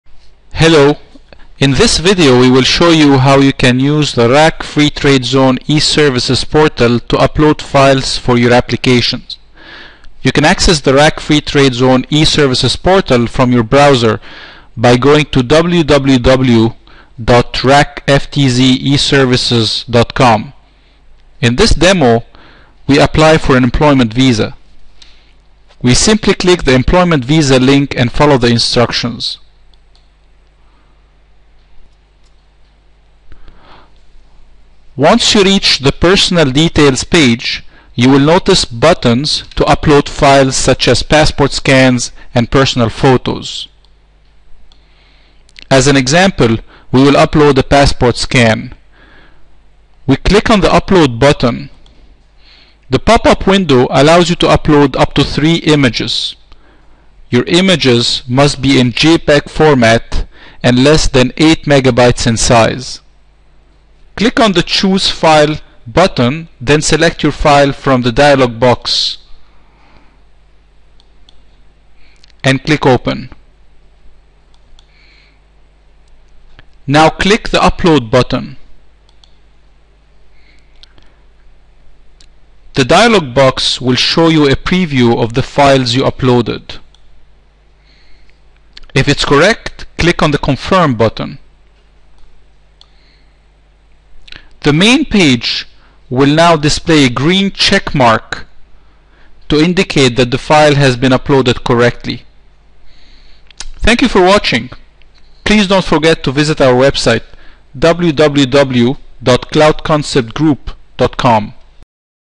RAK FTZ E-services - How to upload files
In this video we will see how you can use the RAK Free Trade Zone e-services portal to upload files for your applications.